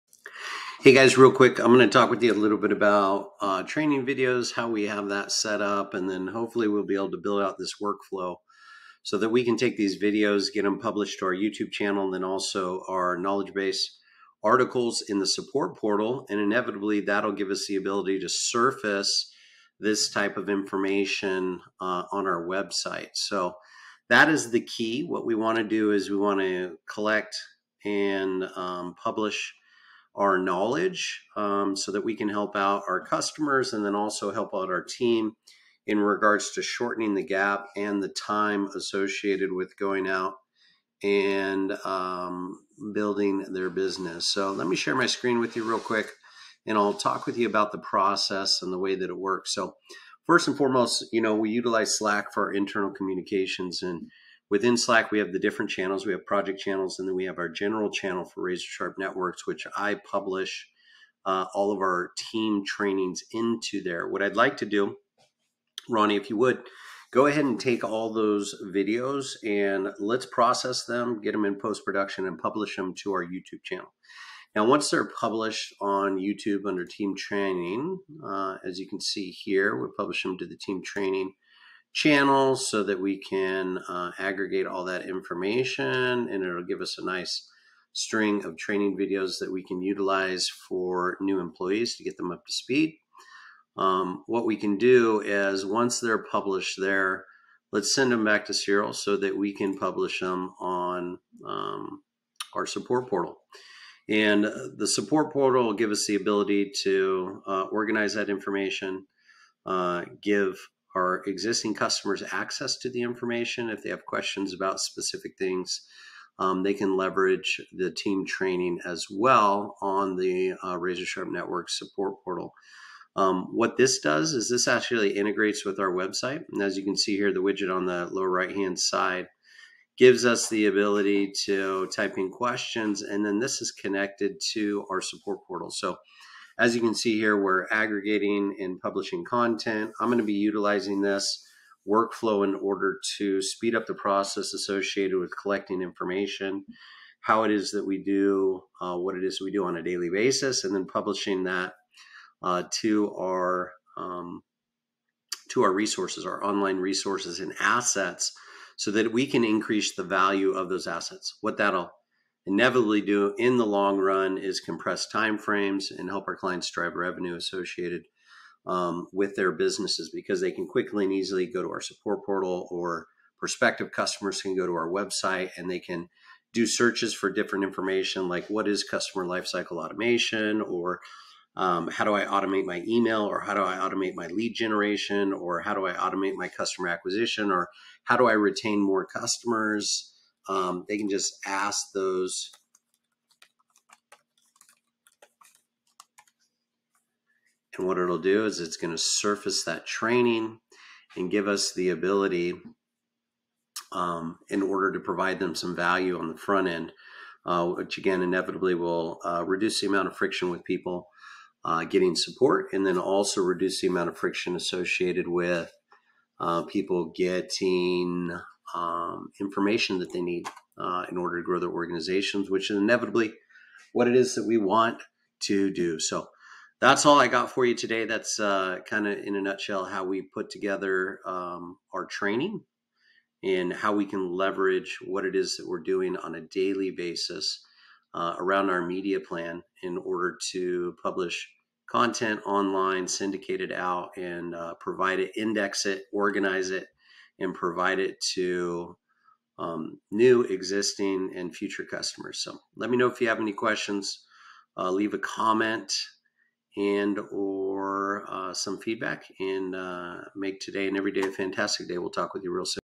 The team training videos - Content syndication
This is how the team training videos are syndicated and published online. How the process works and how it benefits both new and existing customers. The videos are published on YouTube and the support portal. The support portal is connected to the website, which makes it easy for prospective customers to find answers to their questions.

By syndicating the team training videos, we are able to provide value to our customers and help them grow their businesses. This reduces the friction associated with getting support and acquiring information.